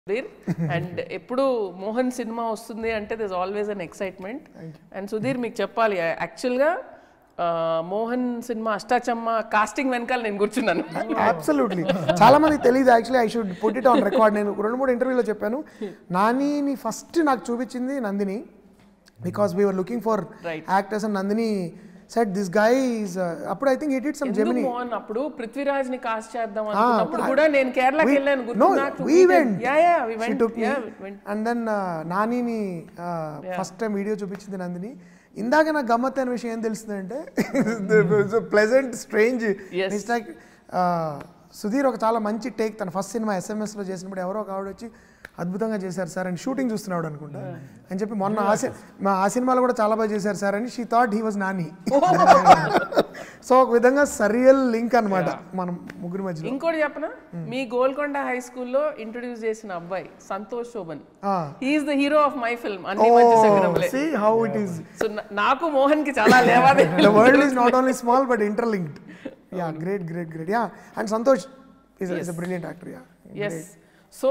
నానిది ఫోన్ లో వీడియో తీసుకొచ్చి చూపించింది | Director Indraganti Mohanakrishna About Nani | CB
Watch : నానిది ఫోన్ లో వీడియో తీసుకొచ్చి చూపించింది | Director Indraganti Mohanakrishna About Nani | CB